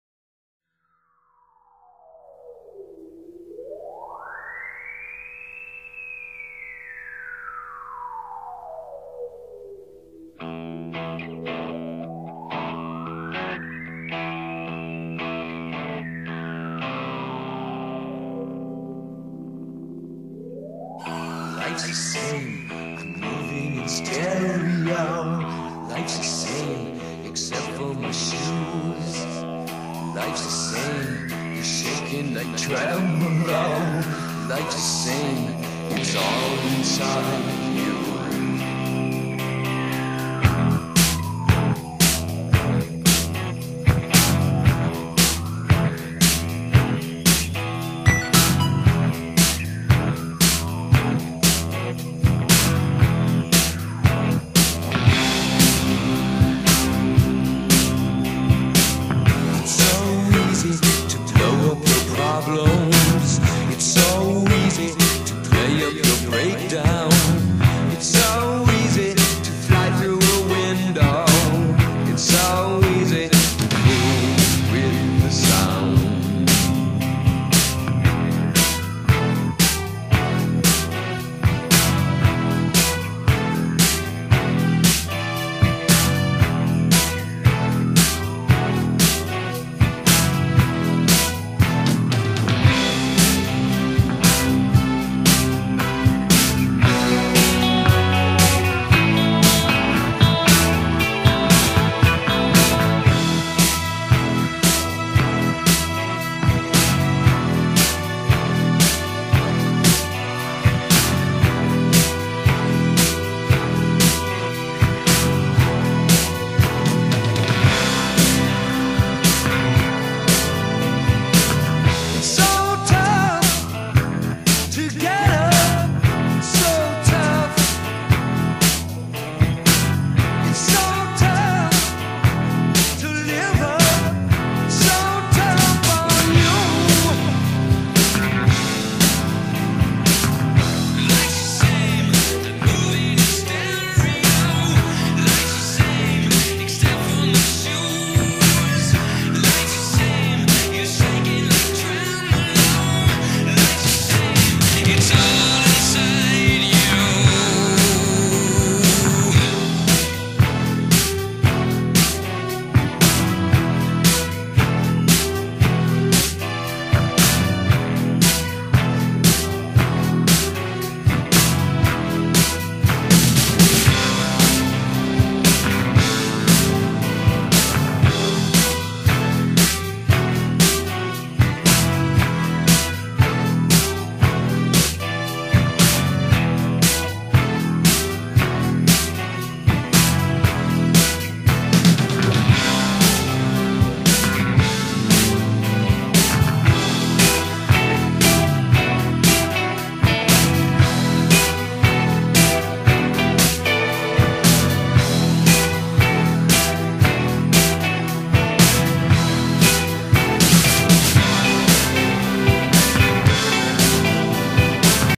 Mountain bike riding lackawanna State Park 7-5-10 part 2
First trip to Lackawanna State Park in 99 degree heat.